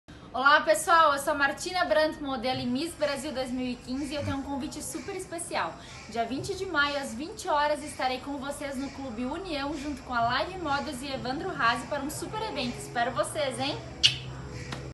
Live Model/ convite especial Marthina Brandt Miss Brasil
Miss Brasil Marthina Brandt
Convida especial para a 5ª Ediçao do Desfile New Face Live
Como grande inspiração para as nossas New Faces que irão desfilar no dia 20 de Maio, convidamos a belíssima miss Brasil lançada pelo nosso instrutor Evandro Hazzy.
A modelo irá realizar o desfile de abertura e também falará um pouco da sua grande trajetória de sucesso.